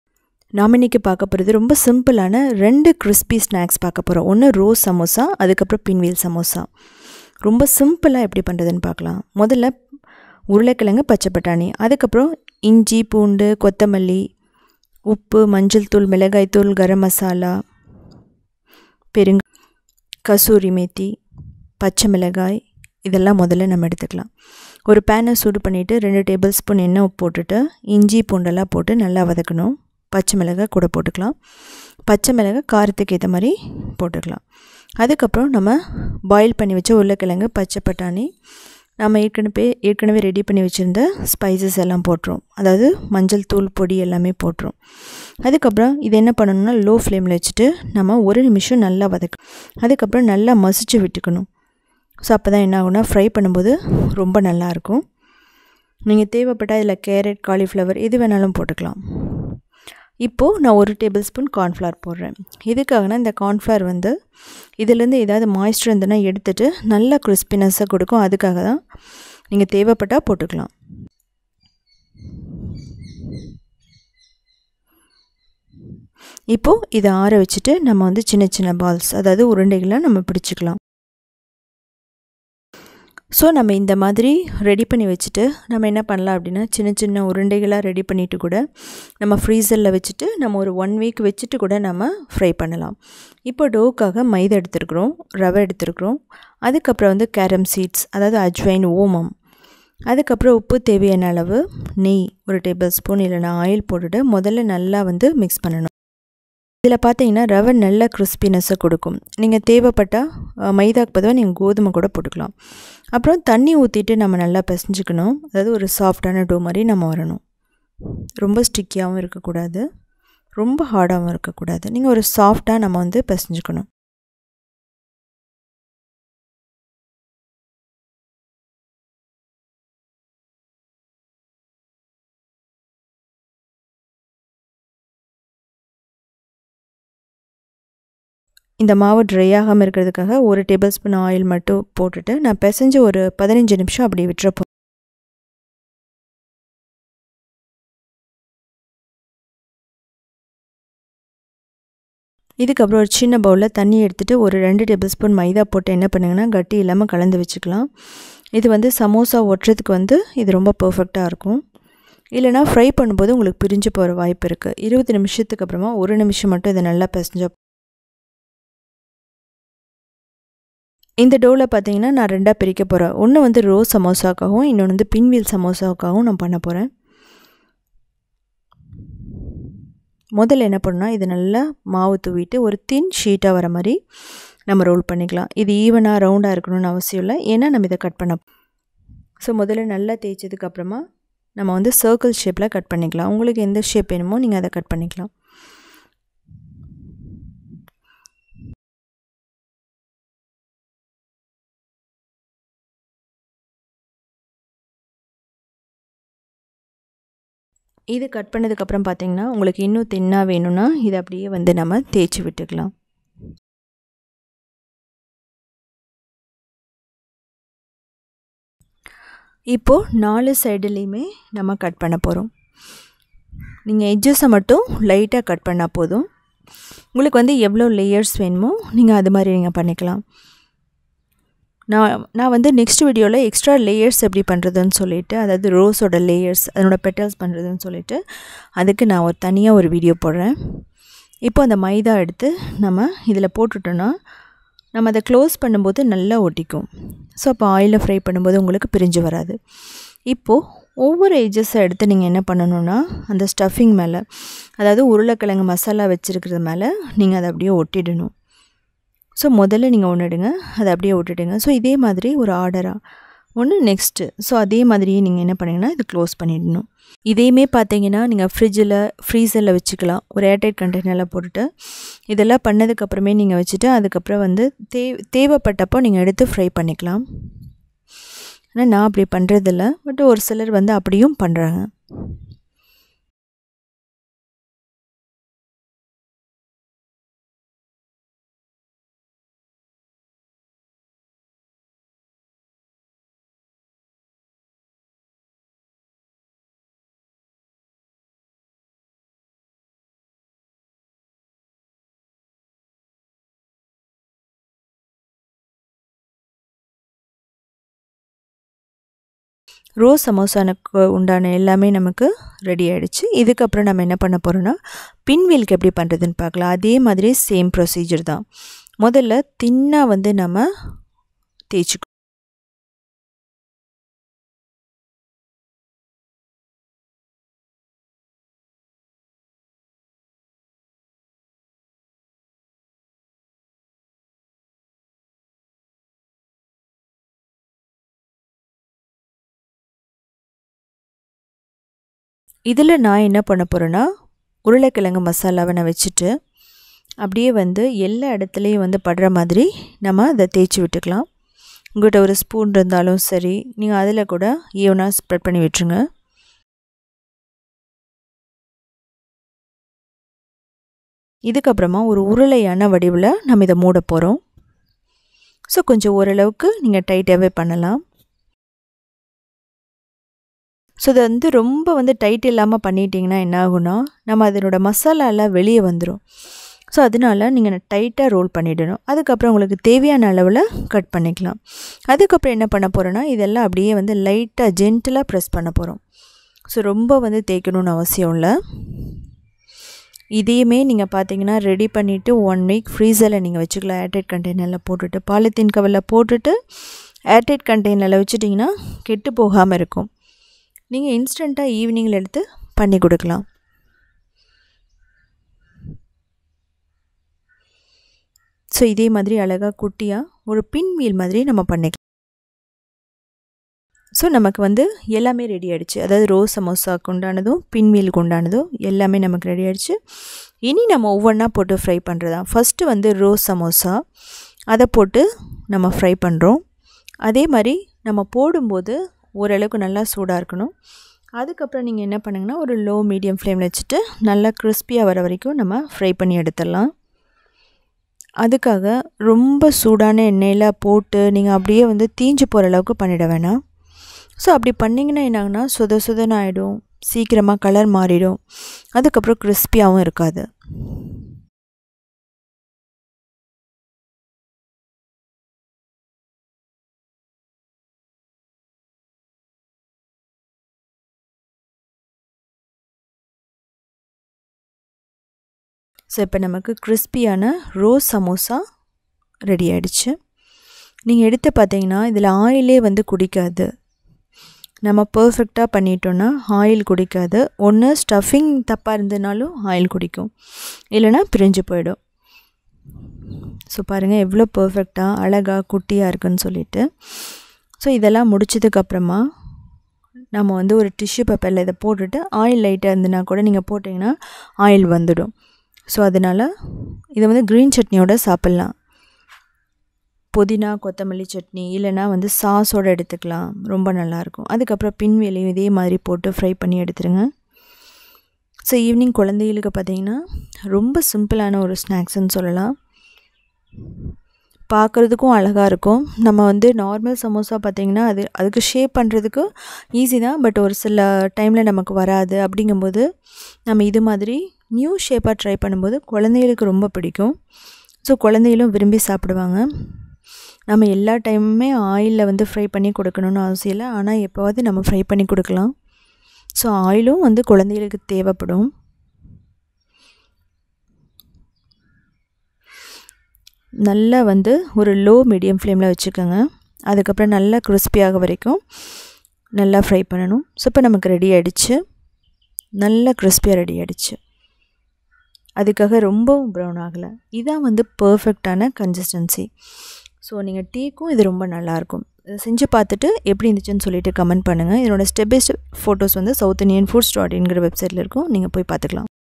Rose samosa & Pinwheel samosa-1 Dough 2 Crispy instant snacks-15 min-New shape-Few ingredient
Rose & Pinwheel samosa is simple, easy to make, instant crispy snack. Only in 15 min. New design & Attractive shape. Only few ingredient.